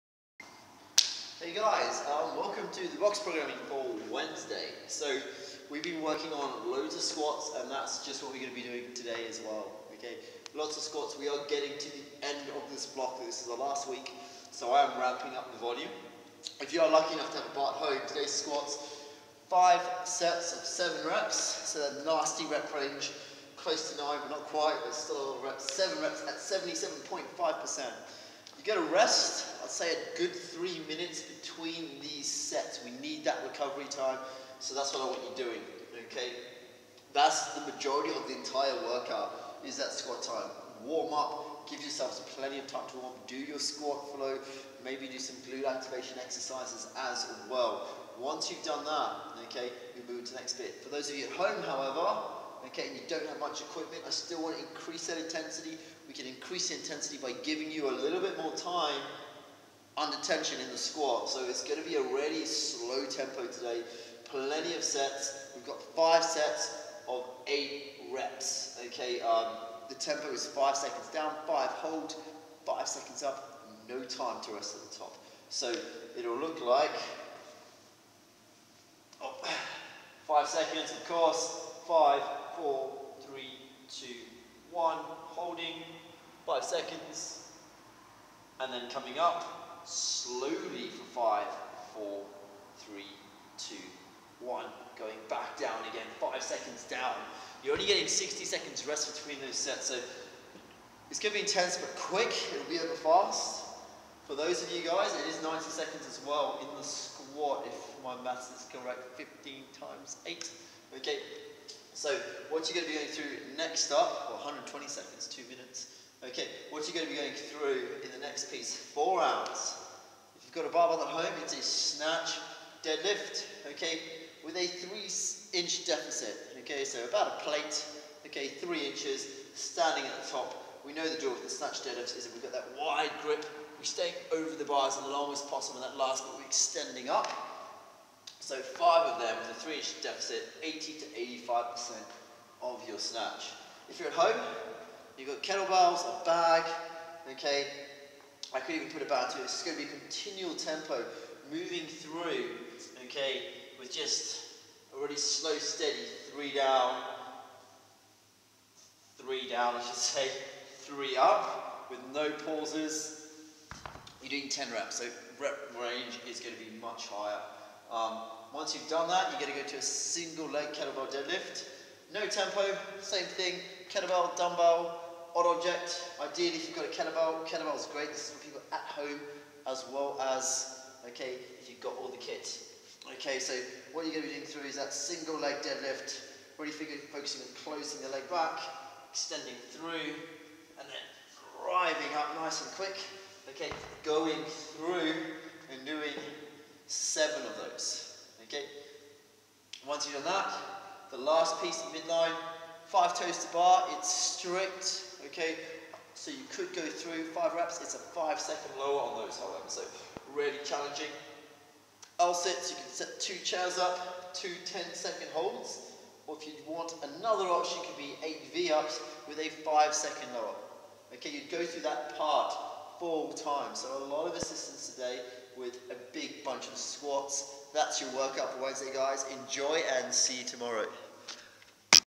Box programming
Back squat free tempo 5x7 at 77.5% rest 3 min between sets
At home option: 5x8 with KB/DB/OO in front rack postion tempo 5/5/5/0 1 min rest

4 rounds
- 5 Snatch DL from a 3' deficit @ 80-85% max snatch
At home option: 10x continuous tempo DL with KB/DB/OO tempo 3/0/3/0
- 7 e.s single leg DL KB/DB/OO
- 5TTB with 5 second lower/2x10 second L sit/ 8 V-up with 5sec lower